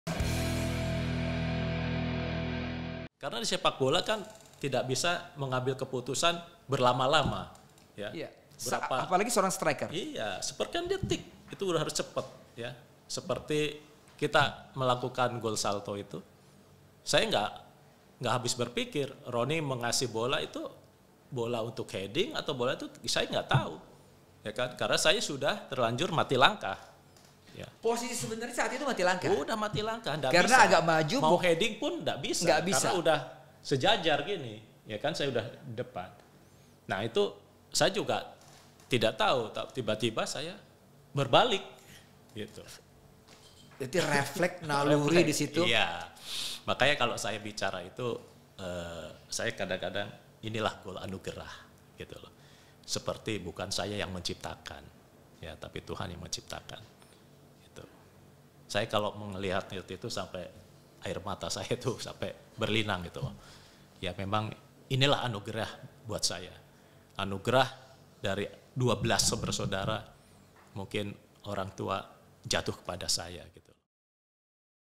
Gol Salto di Piala Asia 1996 Jadi Gol Terbaik, Widodo C Putro Anggap Anugerah
Bagi para penggemar sepak bola, pasti tidak akan pernah lupa dengan momen gol salto yang dicetak oleh Widodo Cahyono Putro, ke gawang Kuwait pada laga Piala Asia 1996. Gol salto tersebut bahkan dinobatkan sebagai gol terbaik Piala Asia dalam voting yang diadakan oleh Federasi Sepak Bola Asia (AFC).